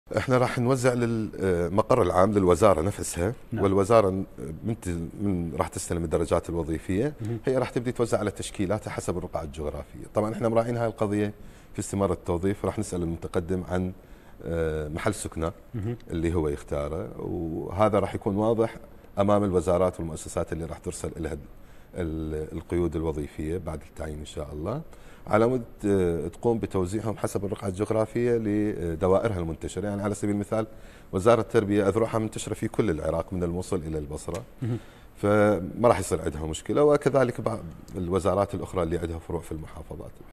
محمود التميمي: تخصيصات وزارة التربية من الدرجات الوظيفية ستعتمد التوزيع الجغرافي
الوظائف في موازنة 2023.. كيف يدير مجلس الخدمة العامة هذا الملف؟ في المحايد مع سعدون ضمد
الضيف: محمود التميمي | رئيس مجلس الخدمة الاتحادي